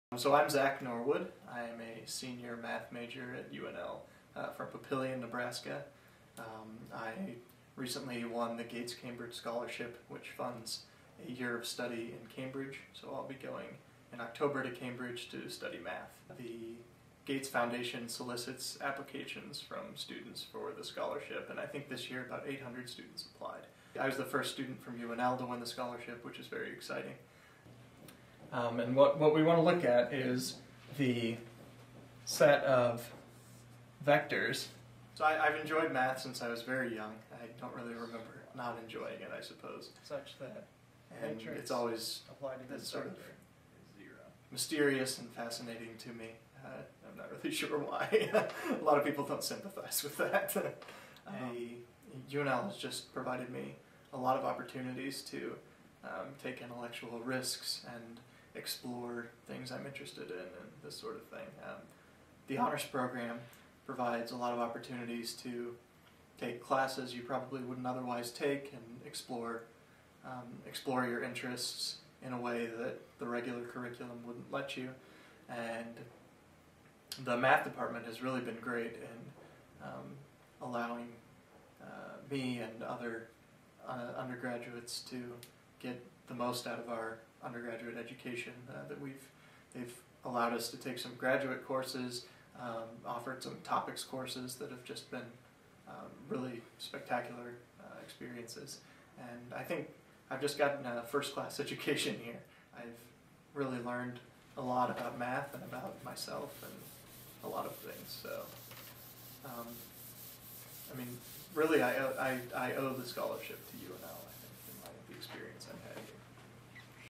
Student's math skills add up to Cambridge scholarship
Originally posted March of 2011